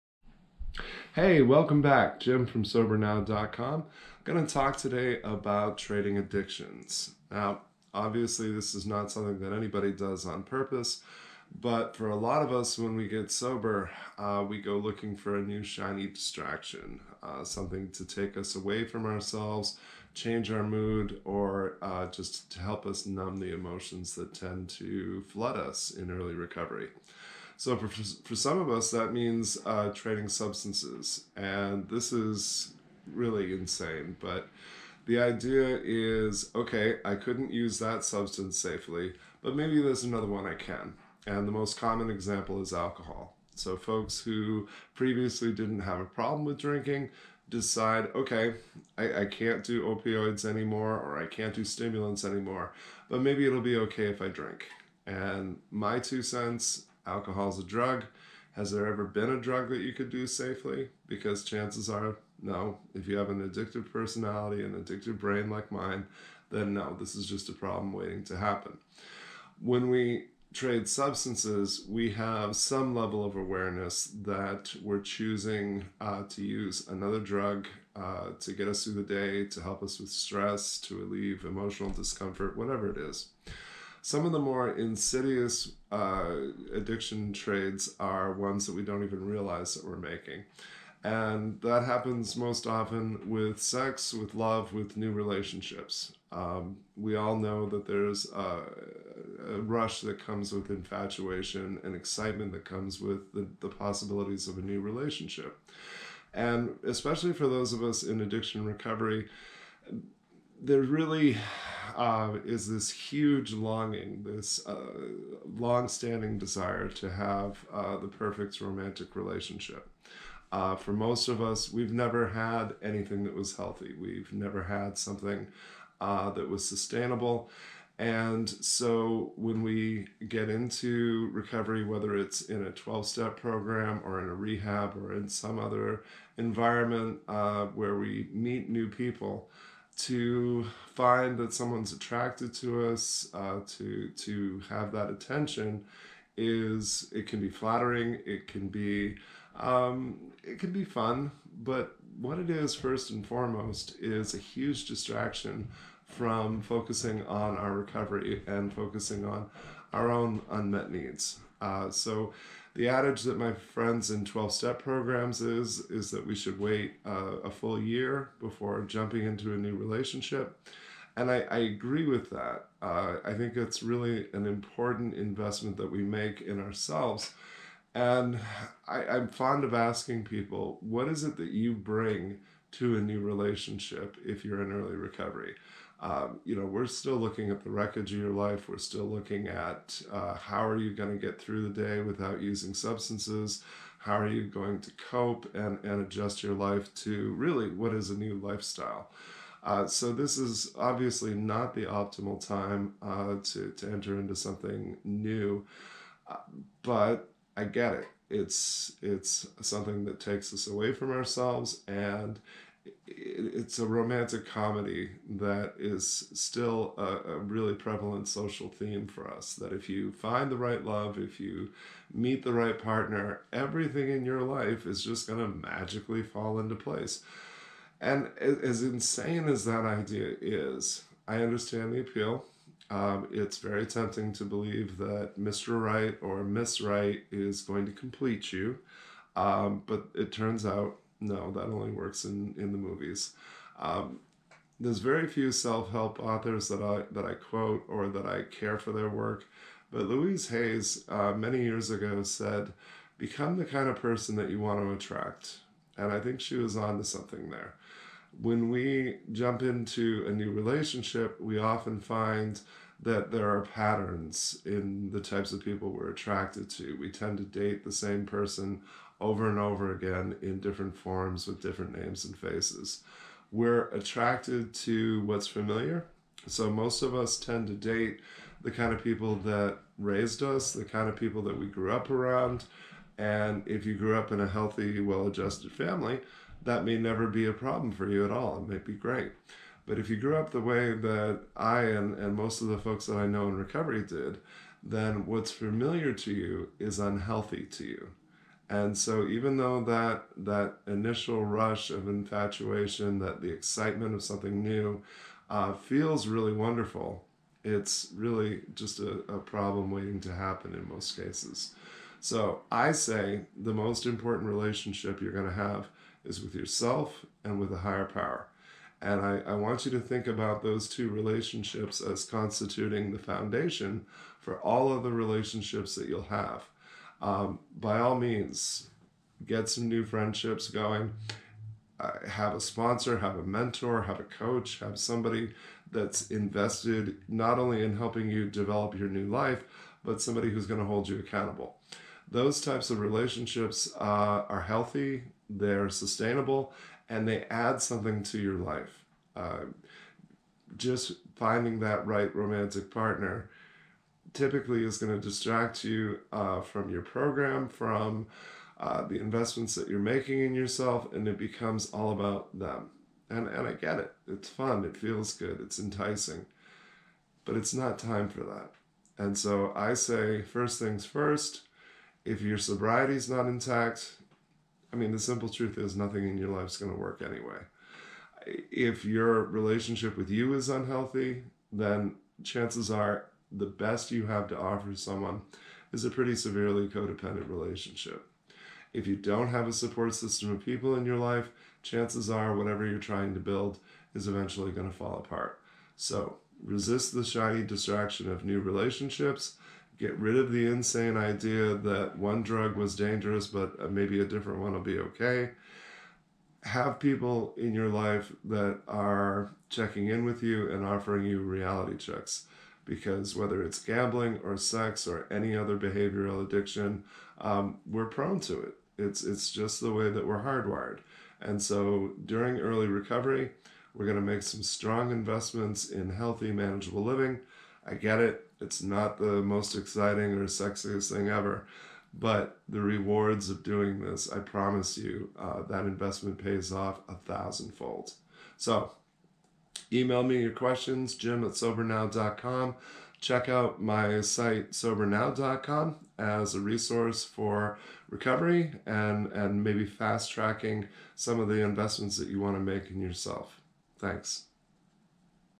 Trading Addictions
It's rarely a conscious choice to trade addictions, so how do we safeguard against it?